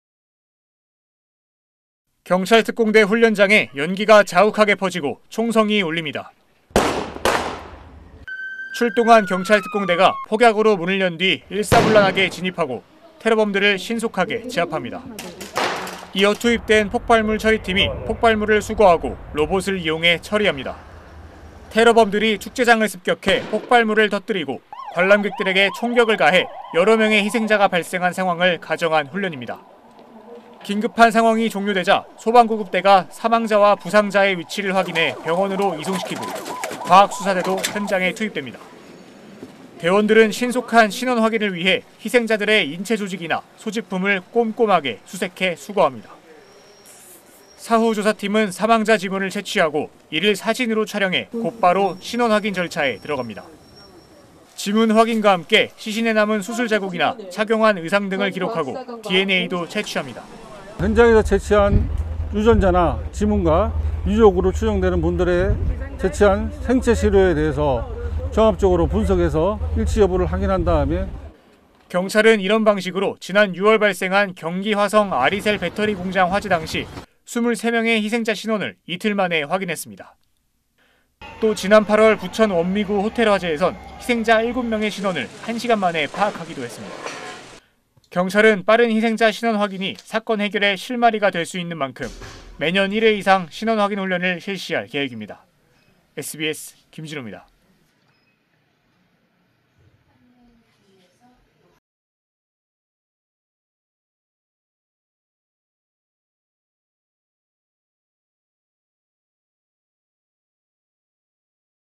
대형 재난 생기면 신속하게…경찰, 신원 확인 훈련 실시 / SBS /
경찰특공대 훈련장에 연기가 자욱하게 퍼지고 총성이 울립니다.

출동한 경찰특공대가 폭약으로 문을 연 뒤 일사불란하게 진입하고, 테러범들을 신속하게 제압합니다.

이어 투입된 폭발물처리팀이 폭발물을 수거하고 로봇을 이용해 처리합니다.

테러범들이 축제장을 습격해 폭발물을 터뜨리고 관람객들에게 총격을 가해 여러 명의 희생자가 발생한 상황을 가정한 훈련입니다.

긴급한 상황이 종료되자 소방 구급대가 사망자와 부상자의 위치를 확인해 병원으로 이송시키고, 과학수사대도 현장에 투입됩니다.

대원들은 신속한 신원 확인을 위해 희생자들의 인체 조직이나 소지품을 꼼꼼하게 수색해 수거합니다.

사후조사팀은 사망자 지문을 채취하고, 이를 사진으로 촬영해 곧바로 신원확인 절차에 들어갑니다.

지문 확인과 함께 시신에 남은 수술 자국이나 착용한 의상 등을 기록하고 DNA도 채취합니다.

[현장에서 채취한 유전자나 지문과 유족으로 추정되는 분들에게 채취한 생채 시료에 대해서 종합적으로 분석해서 일치 여부를 확인한 다음에.]

경찰은 이런 방식으로 지난 6월 발생한 경기 화성 아리셀 배터리 공장 화재 당시 23명의 희생자 신원을 2일 만에 확인했습니다.

또 지난 8월 부천 원미구 호텔 화재에선 희생자 7명의 신원을 1시간 만에 파악하기도 했습니다.

경찰은 빠른 희생자 신원 확인이 사건 해결의 실마리가 될 수 있는 만큼 매년 1회 이상 신원 확인 훈련을 실시할 계획입니다.

(취재 : 김진우, 영상취재 : 설치환, 영상편집 : 원형희, 화면제공 : 경기남부경찰청, 제작 : 디지털뉴스편집부)